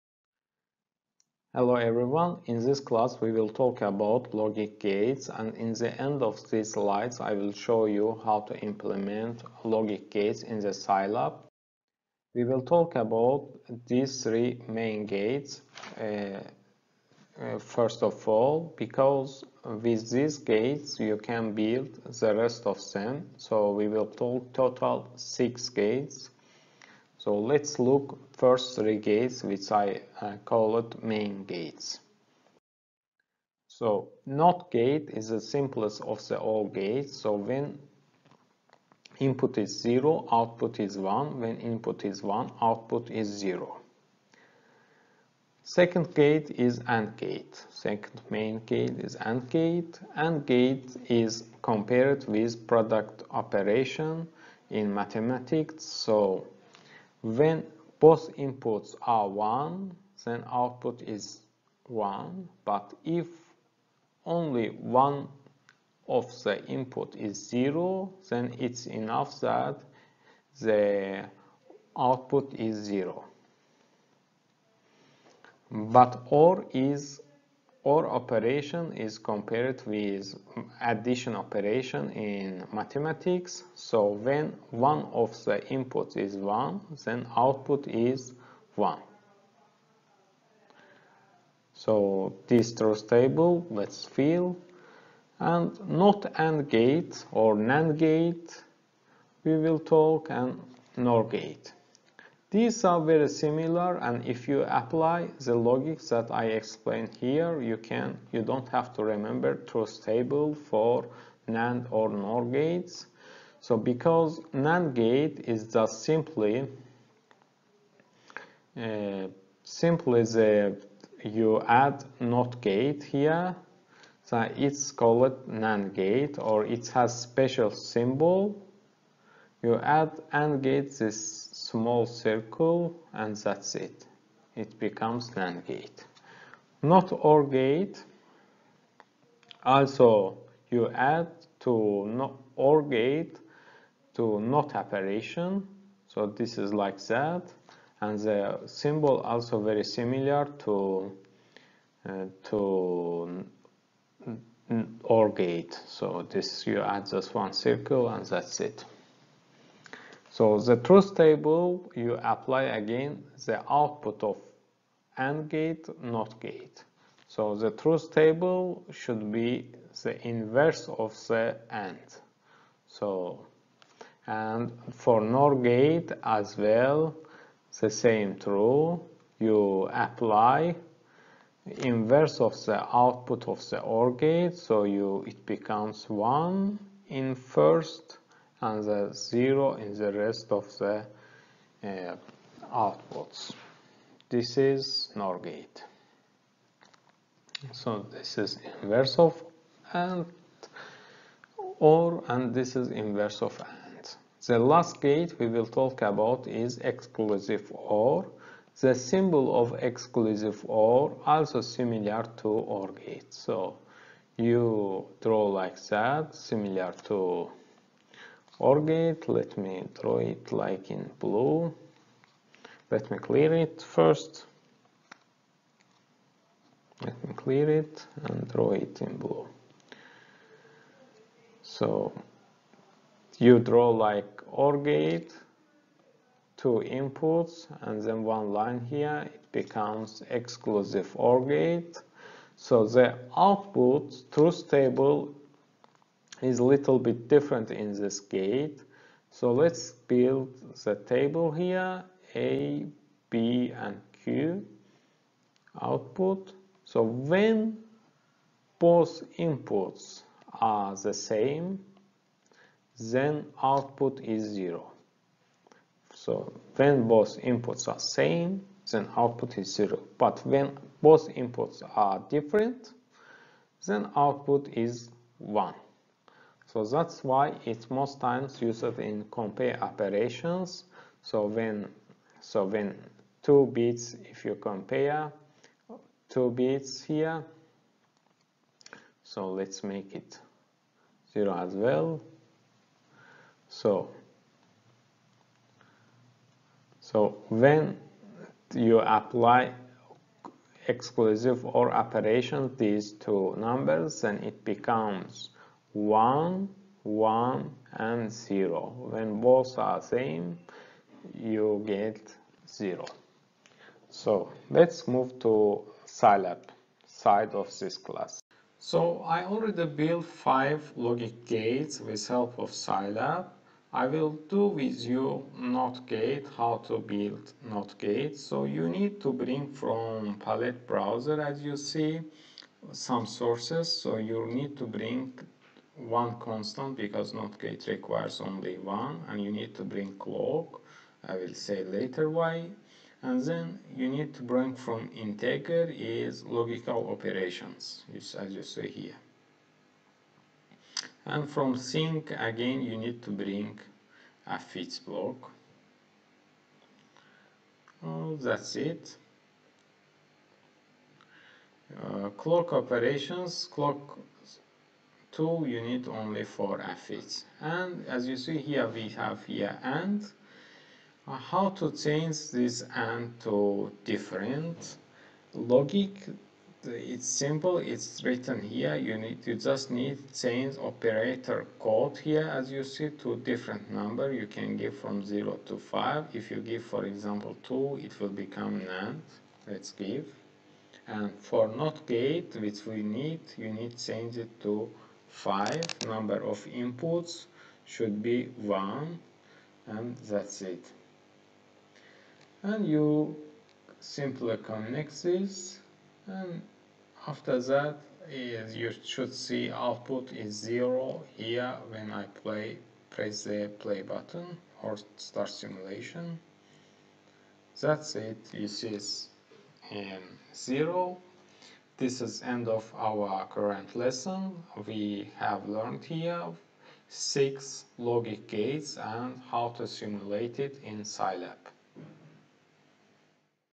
Logic gates simulation with Scilab
In this Scilab class we are talking NOT, AND, OR, NAND, NOR and XOR gates. We show truth table of these and how to simulate logic gates in Scilab.